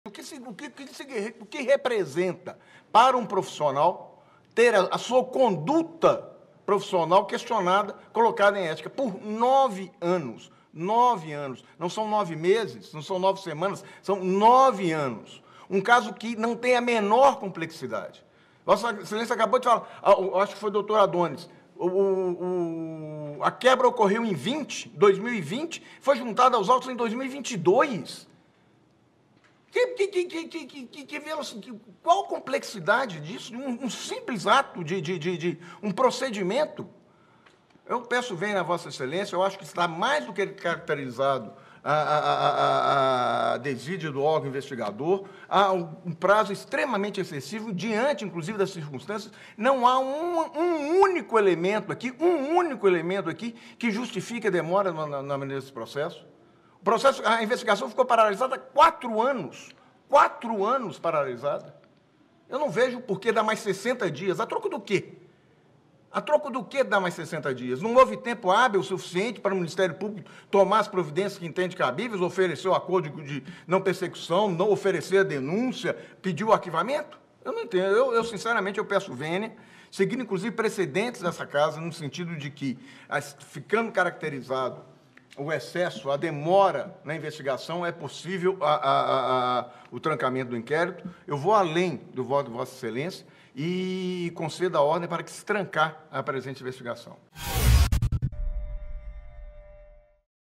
Advogado tem inquérito trancado e chora em voto do ministro Sebastião Reis Jr.
Nesta terça-feira, 16, a 6ª turma do STJ trancou inquérito contra um advogado acusado de se apropriar de valores de sua cliente, uma mulher idosa, analfabeta e economicamente hipossuficiente, nos termos do voto do ministro Sebastião Reis Jr. Enquanto S. Exa. votava, o causídico se emocionou e chorou bastante.